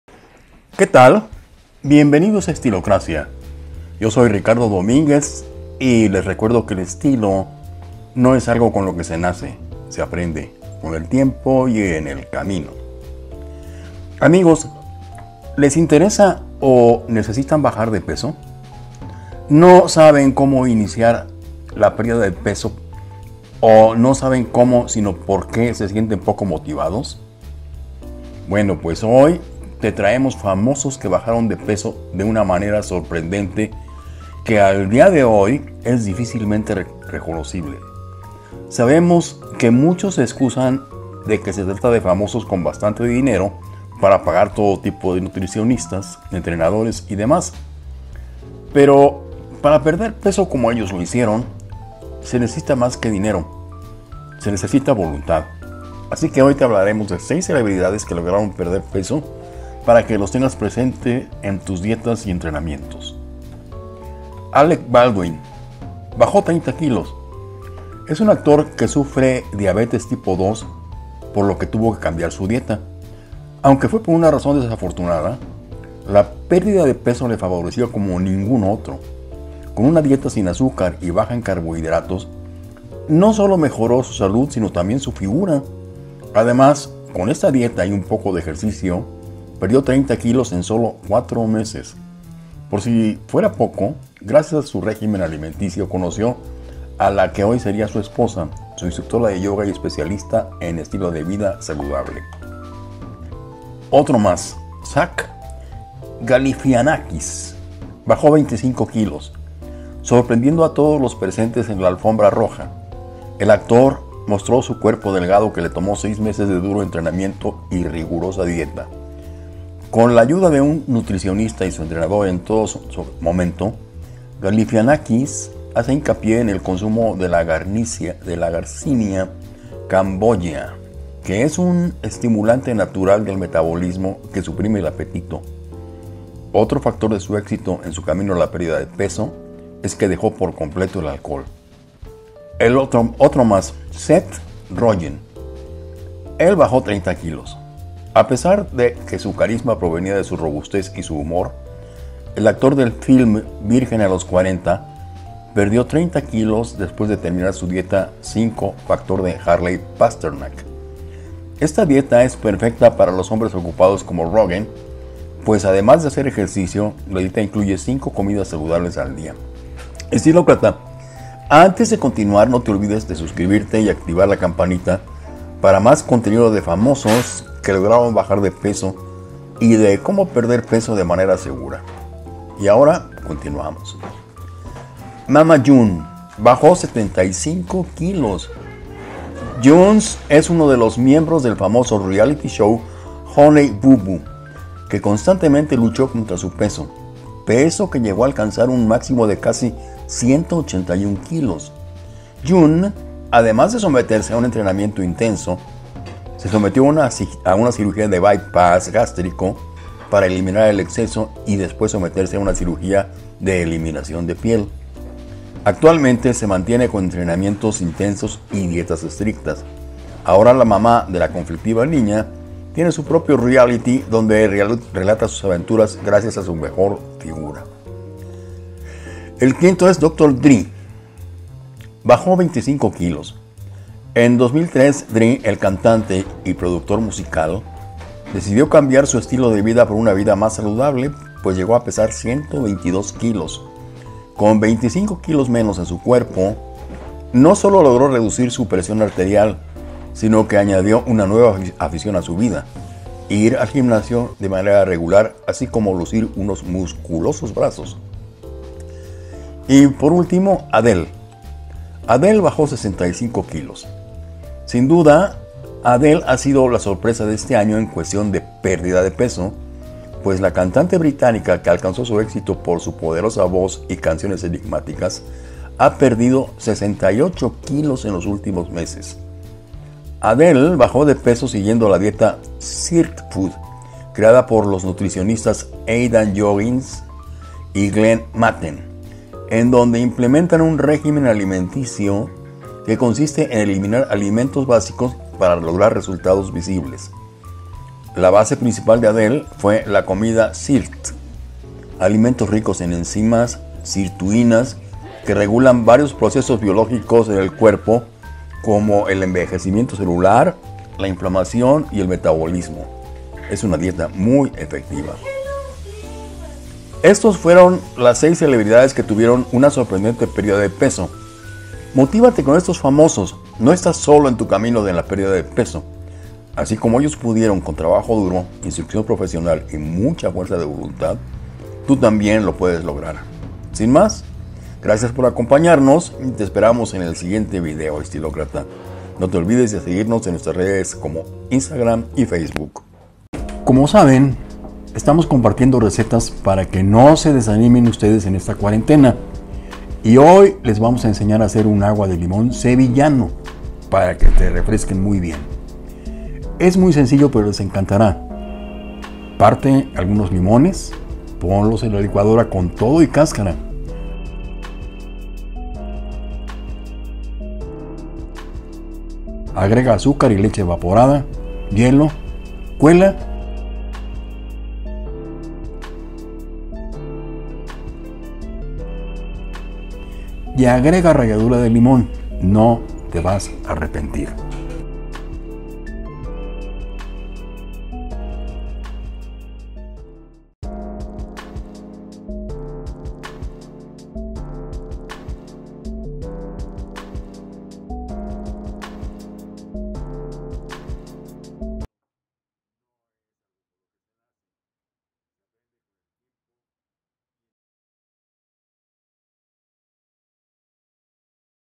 ¿SABÍAS CÓMO SE VEN AHORA? PÉRDIDA DE PESO IMPRESIONANTE! ¿CÓMO LE HICIERON?
ESTOS FAMOSOS LOGRAORN MEJORAR SU FIGURA CON ALGUNAS TÉCNICAS QUE TE COMPARTIMOS AQUÍ. NO TE LO PIERDAS

Visita nuestro sitio asociado de Amazon para productos de tu interés: